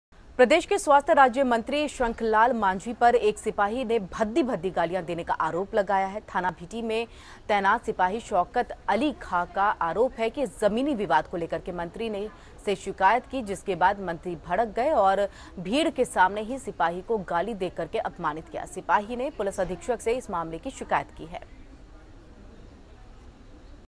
Minister of State for Health Shankh Lal Manjhi accused of abusing UP Police constable
Constable Shauqat  Ali Khan complained Police Superintendent that Minister of State for Health Shankh Lal Manjhi abused him.

Keep watching APN News (Tata Sky 481) as we present latest news and insights.